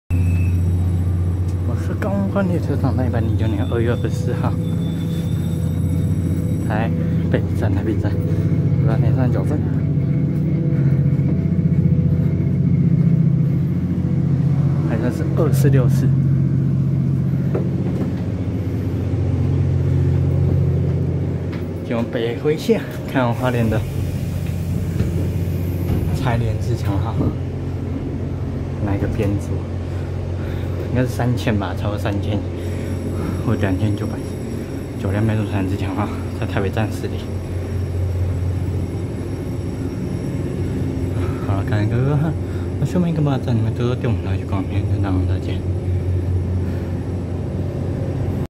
246次經由北迴線開往花蓮的DR3000型九輛編組柴聯自強號
109/2/24台北站18:39分拍攝246次經由北迴線開往花蓮的DR3000型九輛編組柴聯自強號